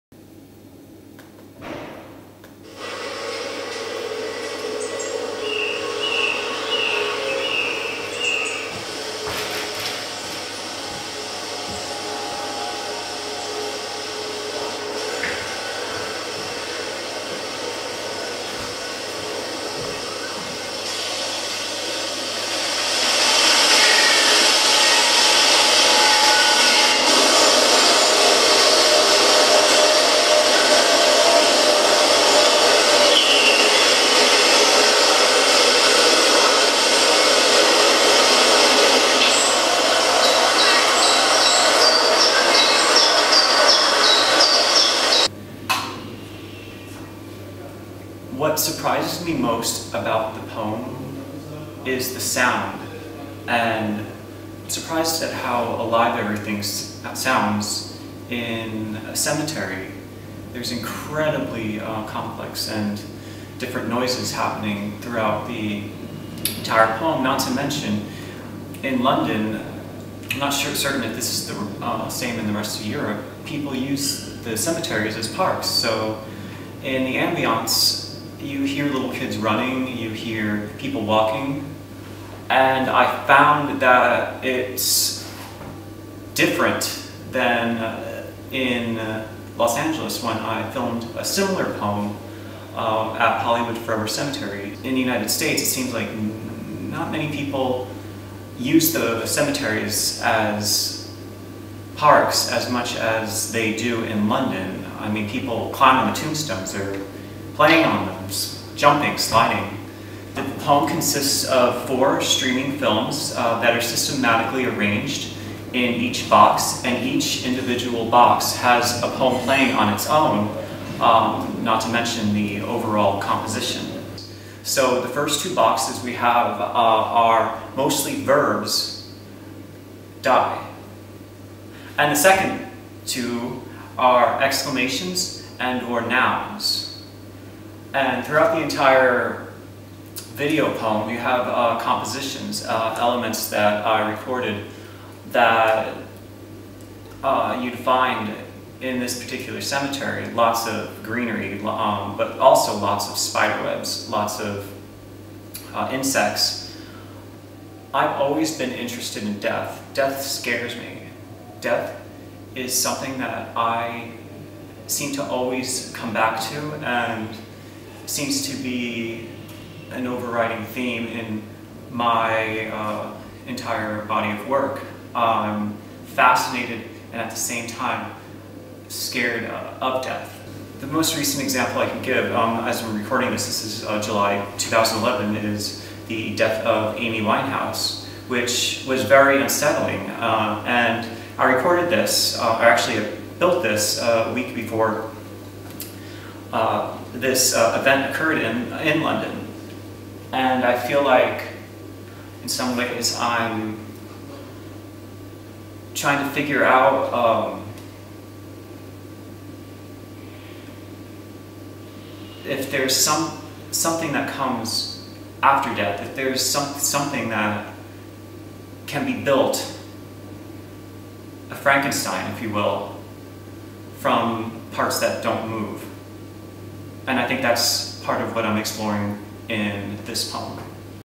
Chris Girard Describes Death Poem
The video poem was filmed at Nunhead Cemetery in London, which is the closest cemetery to where I live. What surprises me most about the poem is the sound - how alive everything sounds when everything else symbolizes death; tombstones broken and underneath overgrown plants. I used the video function of a Canon 7D to film spiderwebs, dead leaves and flies, anything that was within the cemetery, including, of course, tombstones. The poem consists of four streaming films that are systematically arranged into a box, which create a larger poem when words are juxtaposed next to each other in the larger box.

While the constraint of place and filming objects that symbolize death structure the poem, each individual poem uses words that compositionally and grammatically fit into the area of the box it streams in. Verbs, primarily, and objects that visually correspond to the top images are placed on the top two boxes. Nouns, exclamations and other objects that visually correspond to the bottom images are placed in the bottom two boxes to end the poem.